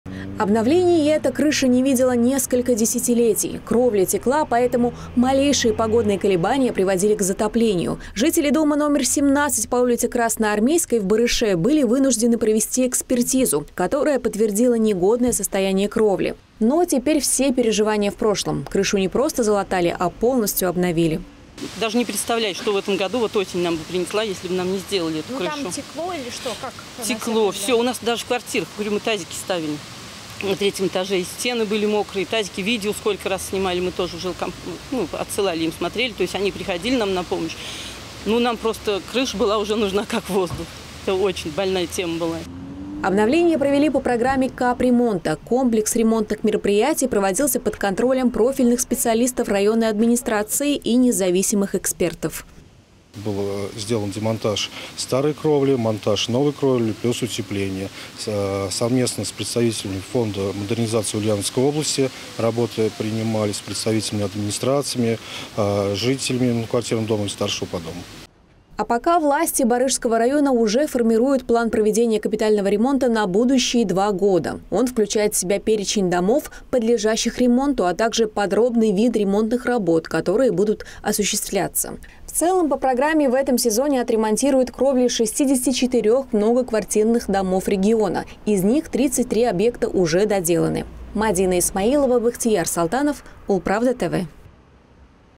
Крыша в порядке. Где и как в Ульяновской области капитально ремонтируют дома
В Барышском районе обновляют многоквартирные дома. Под региональную программу капитального ремонта в текущем году попало 6 домов. Четыре из них - в самом районном центре, и два - в посёлке имени Ленина. Съемочная группа побывала в доме номер 17 на Красноармейской улице.